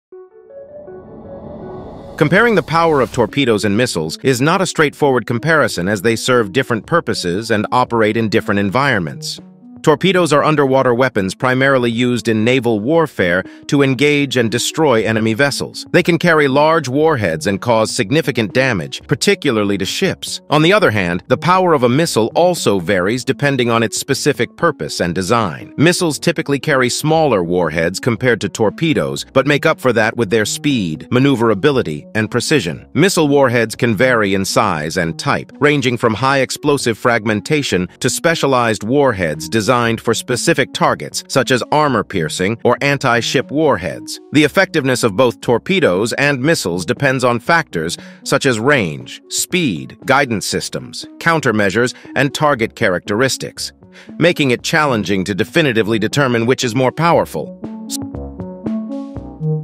Are torpedoes more powerful than missiles?
Comparing the power of torpedoes and missiles is not a straightforward comparison as they serve different purposes and operate in different environments. Torpedoes are underwater weapons primarily used in naval warfare to engage and destroy enemy vessels. They can carry large warheads and cause significant damage, particularly to ships. On the other hand, The power of a missile also varies depending on its specific purpose and design. Missiles typically carry smaller warheads compared to torpedoes but make up for that with their speed, maneuverability, and precision. Missile warheads can vary in size and type, ranging from high-explosive fragmentation to specialized warheads designed for specific targets, such as armor-piercing or anti-ship warheads.

The effectiveness of both torpedoes and missiles depends on factors such as range, speed, guidance systems, countermeasures, and target characteristics, making it challenging to definitively determine which is more powerful.